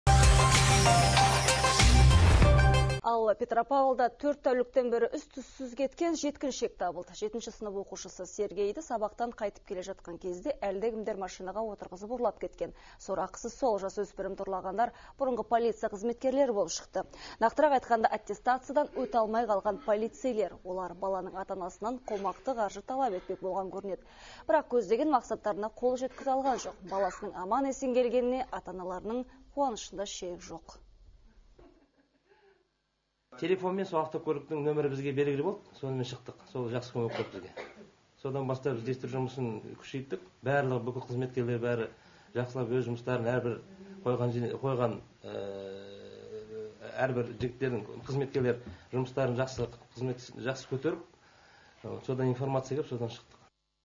Ұрланған бала табылды
Петропавлда 4 тәуліктен бері із түссіз кеткен жеткіншек табылды. Жетінші сынып оқушысы Сергейді сабақтан қайтып келе жатқан кезде әлдекімдер машинаға отырғызып, ұрлап әкеткен. Сорақысы сол, жасөспірімді ұрлағандар бұрынғы полиция қызметкерлері болып шықты.